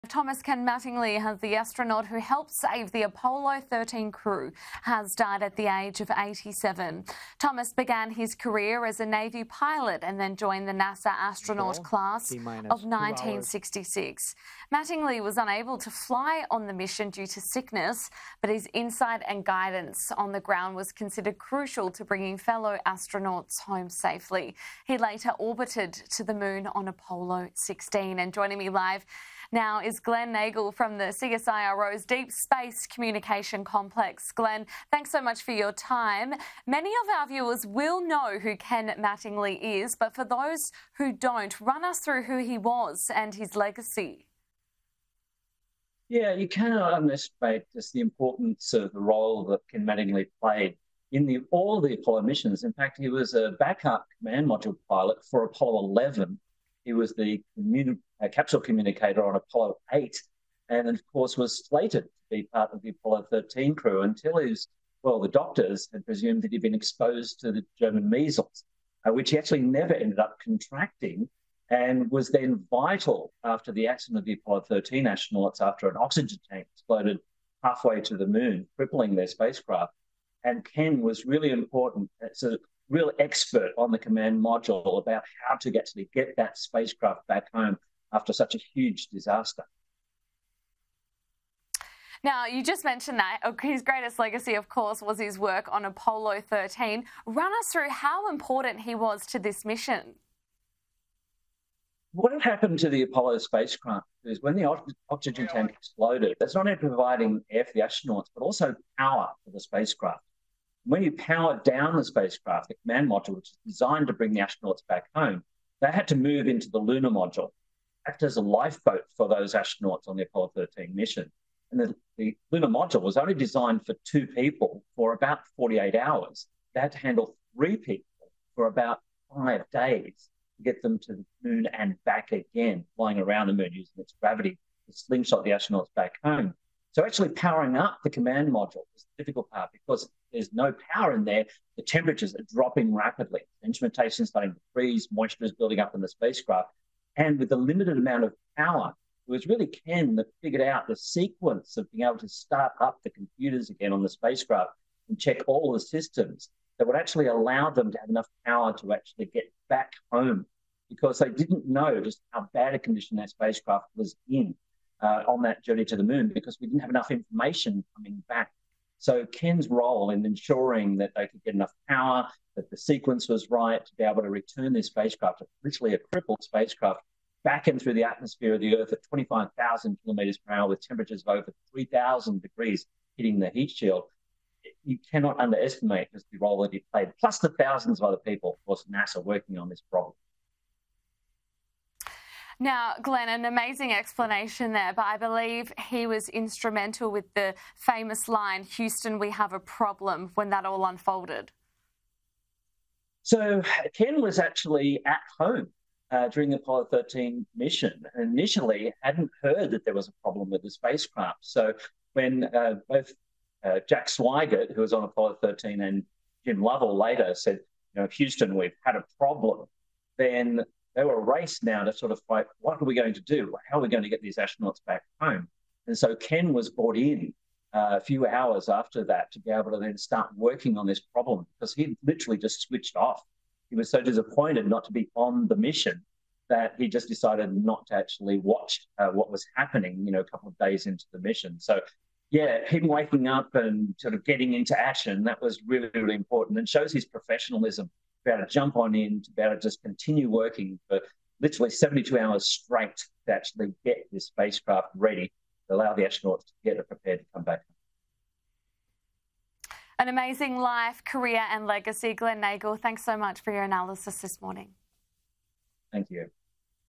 Apollo astronaut Thomas Ken Mattingly dies at age 87
Apollo astronaut Thomas Ken Mattingly, known for helping save the Apollo 13 crew, has died at the age of 87.

Thomas began his career as a Navy pilot and then joined the NASA astronaut class of 1966.

Mattingly was unable to fly on the Apollo 13 due to sickness.

However, his insight and guidance on the ground were considered crucial to bringing fellow astronauts Jack Swigert, Fred Haise and James Lovell home safely.

He later orbited the moon on Apollo 16.